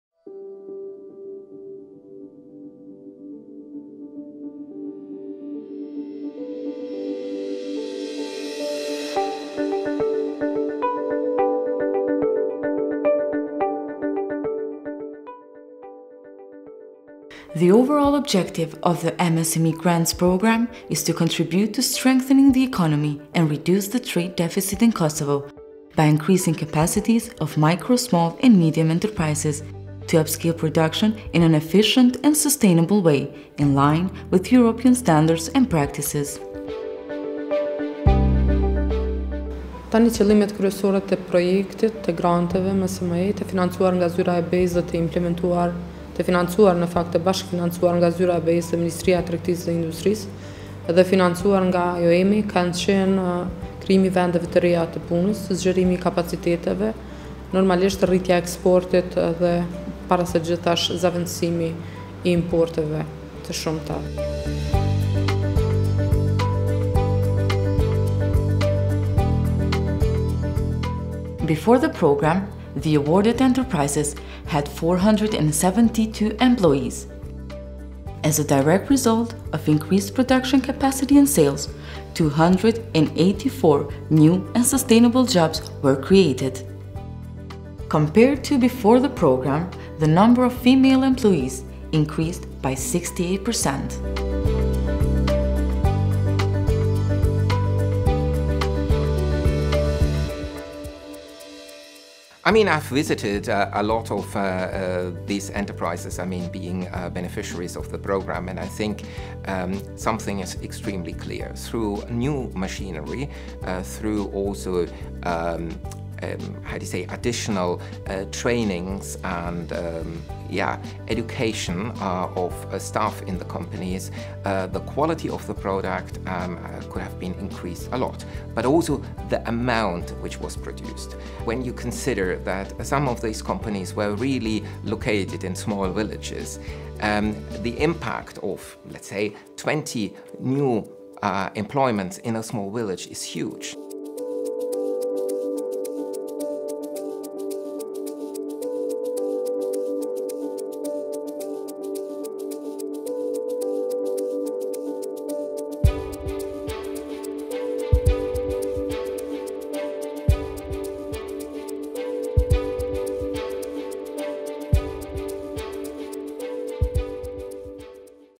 Sustainable Employment – MSME Grants Programme, IOM Kosovo
This short documentary describes the MSME Grants Programme in Kosovo (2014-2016) that increased the capacities of 36 micro, small and medium enterprises in Kosovo to upscale production in an efficient and sustainable way.  As a result, the 36 supported enterprises increased exports, produced more good locally to replace the need for imports, and in the process, created 284 new and sustainable jobs.

The MSME Grants Programme was financed by the European Union Office in Kosovo and the Ministry of Trade and Industry, and implemented by IOM Kosovo.